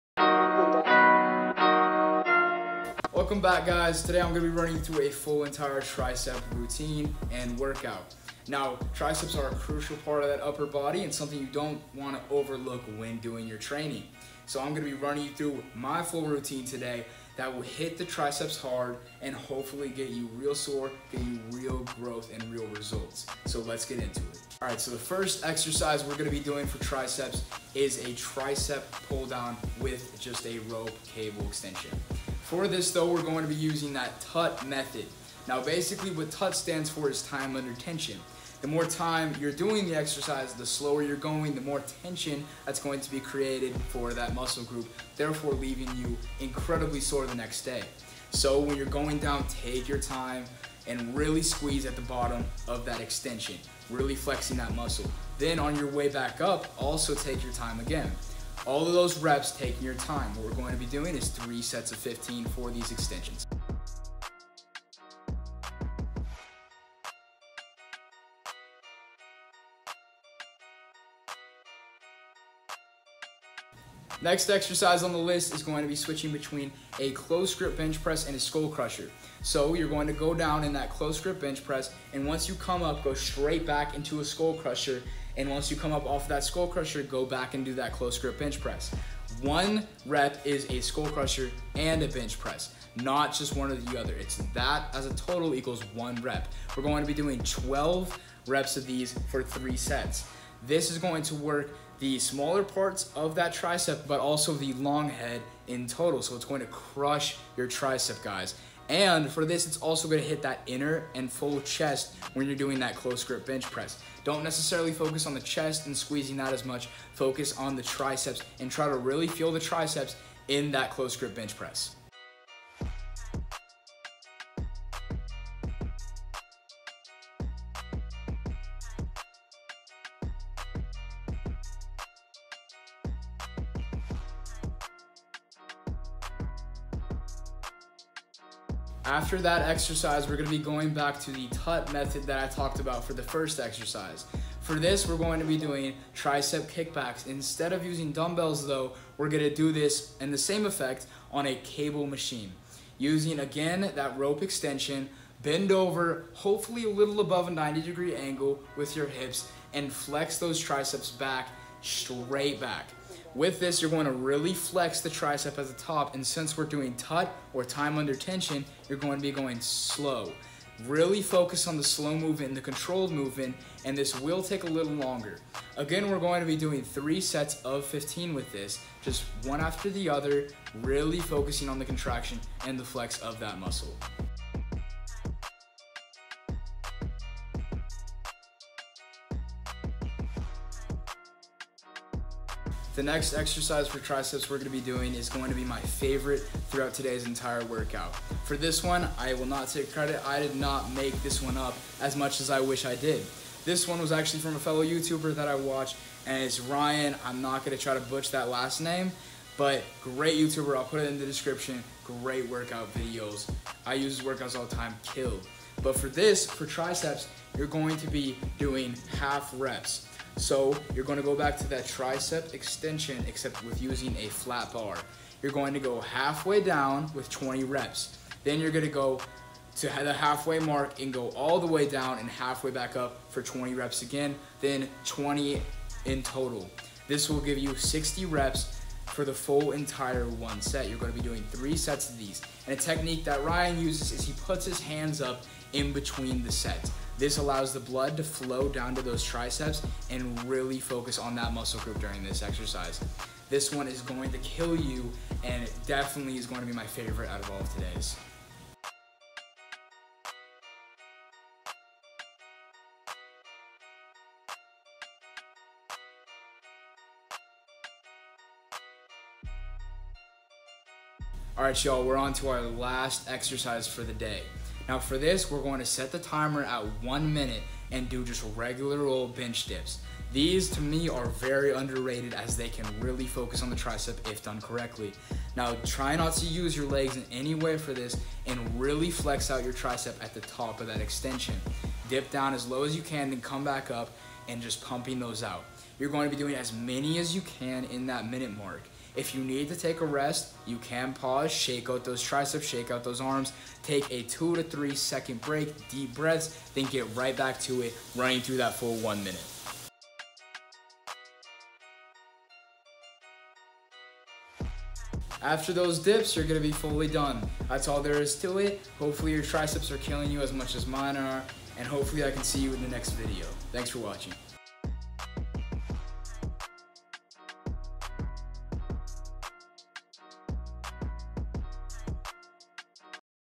The Only Tricep Workout You Need To Be Doing As A Teen!
Welcome to today’s video everyone. We’re gonna be absolutely demolishing our triceps with some new techniques that you may have never seen before. We’re also gonna be using TUT, which means Time Under Tension,  this will give us  slower, more contracted movements for a better work.  :)

Workout for today;

Rope extension pull downs (TUT)

Skull crushed and close grip bench press switch off

Pulley Kickbacks

Intensity Pushdowns

One minute of Dips